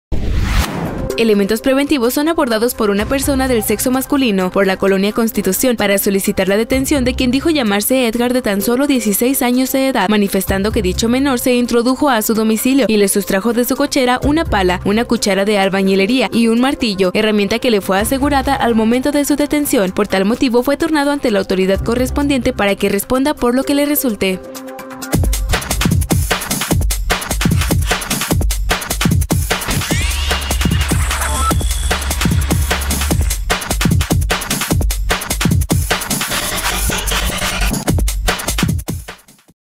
Policíaca | Menor detenido por presunto robo
Elementos preventivos son abordados por una persona del sexo masculino, por la colonia Constitución, para solicitar la detención de quien dijo llamarse Edgar de tan solo 16 años de edad, manifestando que dicho menor se introdujo a su domicilio y le sustrajo de su cochera una pala, una cuchara de albañilería y un martillo, herramienta que le fue asegurada al momento de su detención.